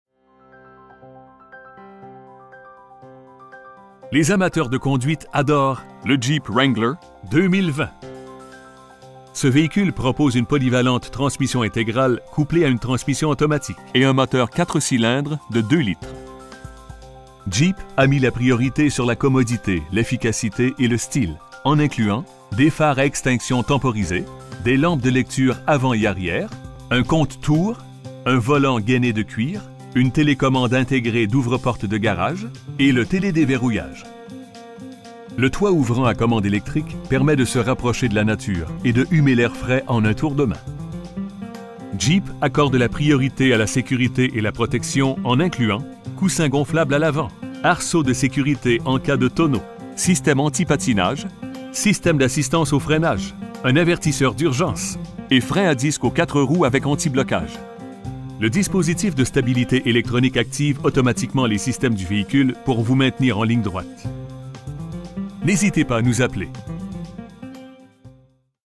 2020 Jeep Wrangler Unlimited Sahara North
Les amateurs de conduite adorent le Jeep Wrangler 2020. Ce véhicule propose une polyvalente transmission intégrale couplée à une transmission automatique et un moteur 4 cylindres de 2,0 L. Jeep a mis la priorité sur la commodité, l'efficacité et le style en incluant: des phares à extinction temporisée, des lampes de lecture avants et arrières, un compte-tours, un volant gainé de cuir, une télécommande intégrée d'ouvre-porte de garage, des sièges baquets à l'avant, et le télédéverrouillage. Le toit ouvrant à commande électrique permet de se rapprocher de la nature et d'humer l'air frais en un tour de main. Jeep accorde la priorité à la sécurité et la protection en incluant: coussins gonflables à l'avant, arceau de sécurité en cas de retournement, système antipatinage, système d'assistance au freinage, un avertisseur d'urgence, et freins à disque aux 4 roues avec antiblocage. Le dispositif de stabilité électronique active automatiquement les sytèmes du véhicule pour vous maintenir en ligne droite. Ce qui nous rend fiers, c'est vous offrir uniquement des véhicules de grande qualité. N'hésitez pas à nous appeler.